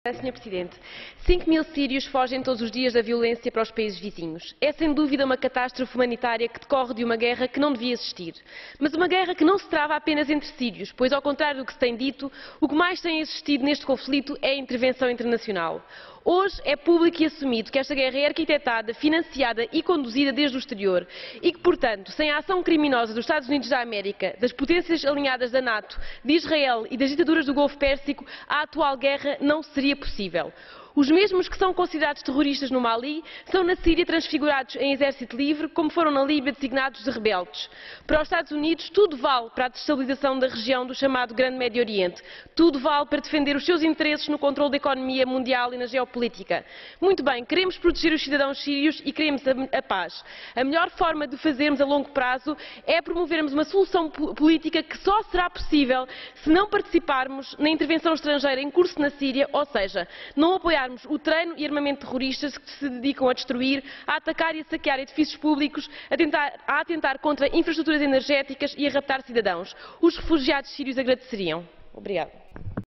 A Síria está a ser agredida pelos EUA e os seus aliádos e satélites
Inês Zuber, PCP, março, 21013